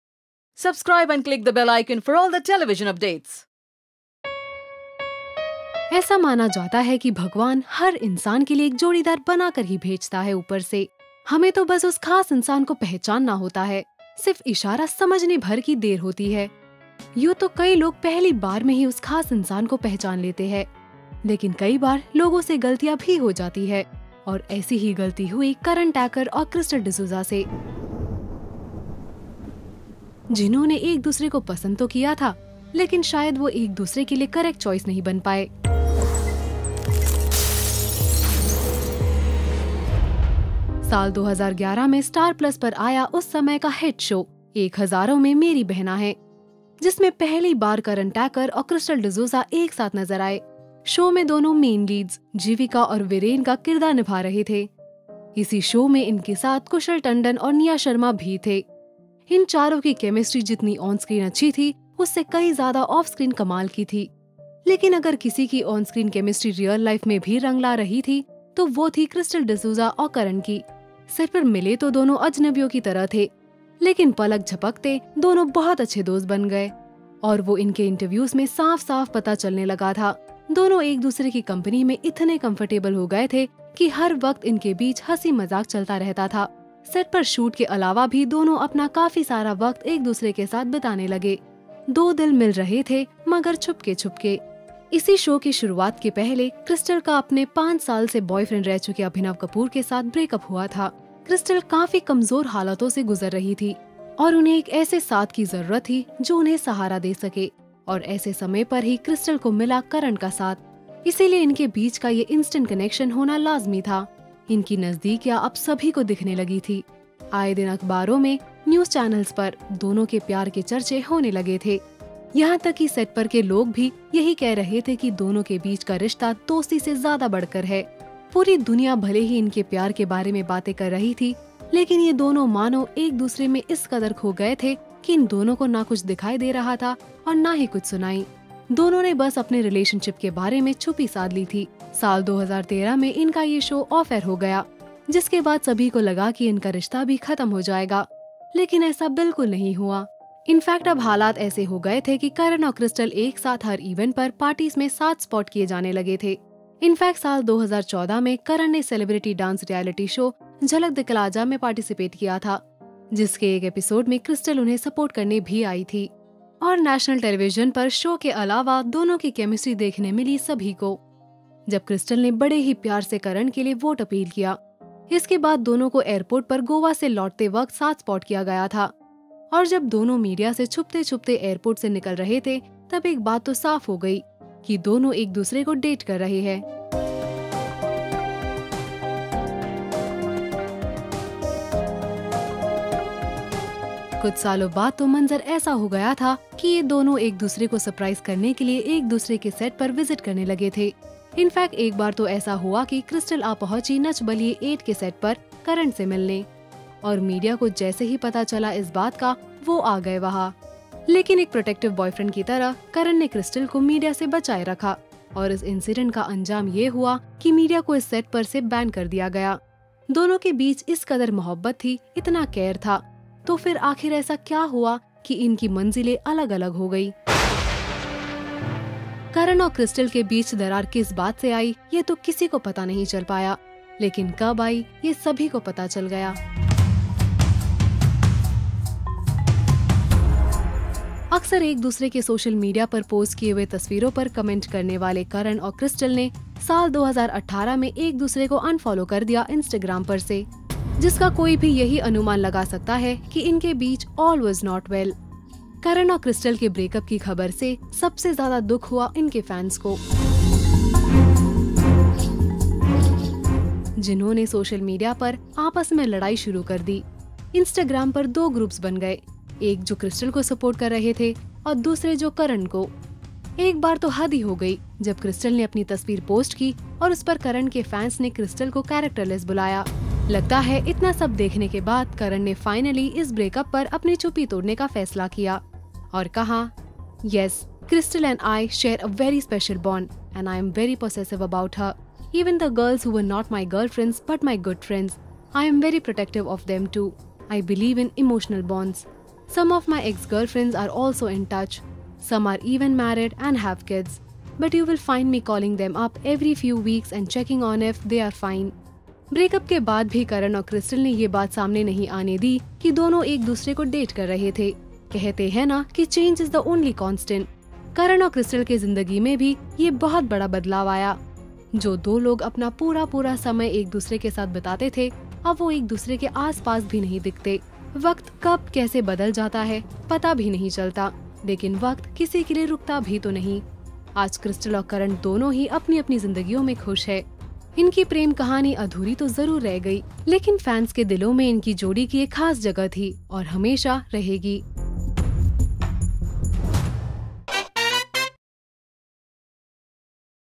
Krystle D'souza & Karan Tacker | Break Up Story | Humari Adhuri Kahani 2
Krystle Dsouza & Karan Tacker were one of the most talked about Television couple. But all of a sudden their Break up shocked us all. see their full story on Hamari Adhuri Kahani.